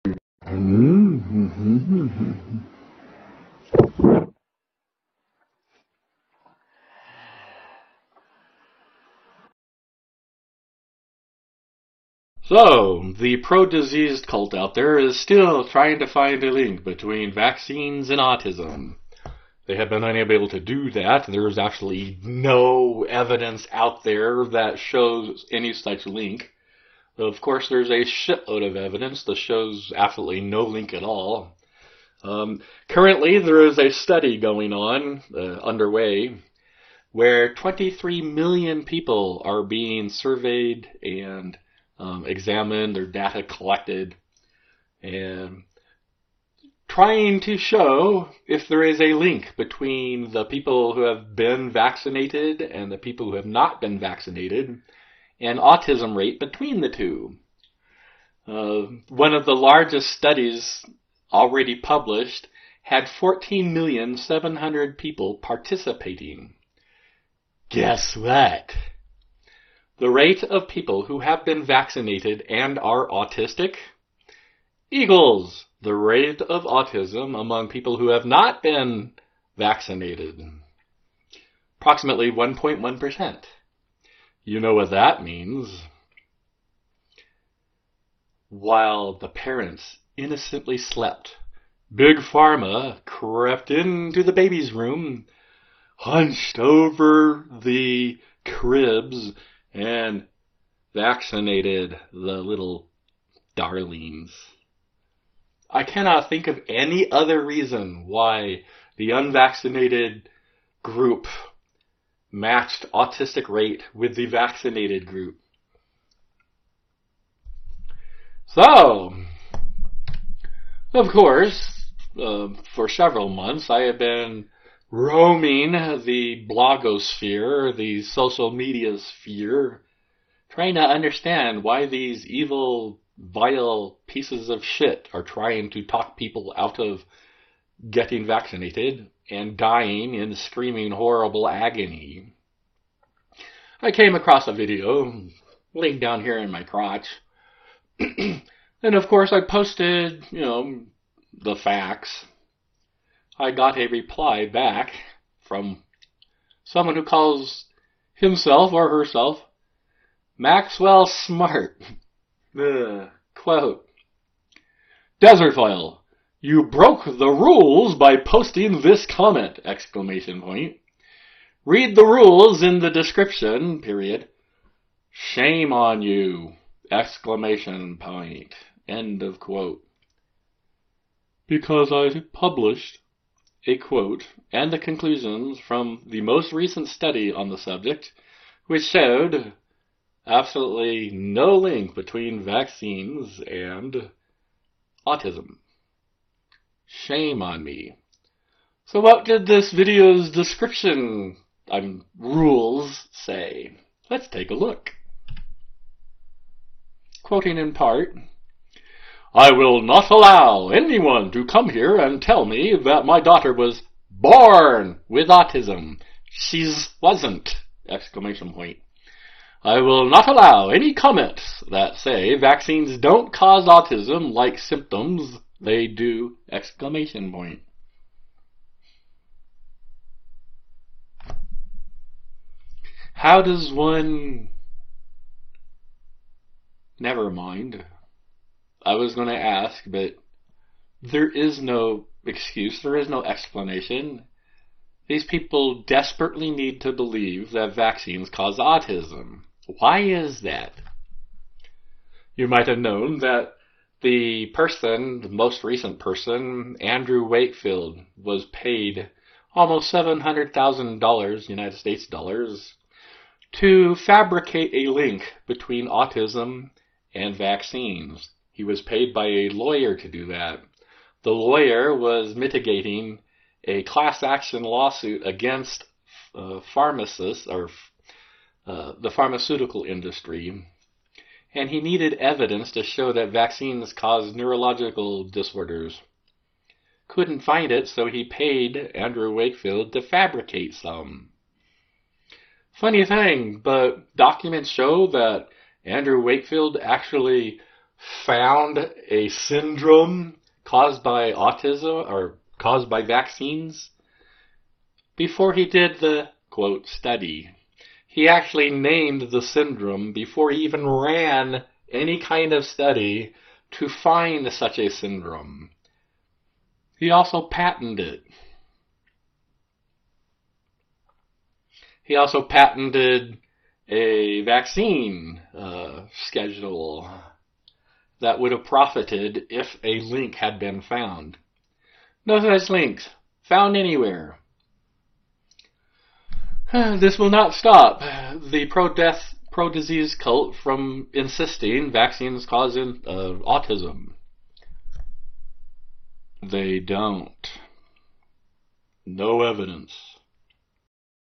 BOMBSHELL! Handley and Wakefield vindicated! CDC admits truth about vaccines and autism!
Surprise, surprise! Andrew Wakefield was paid by lawyers to undermine the MMR vaccine: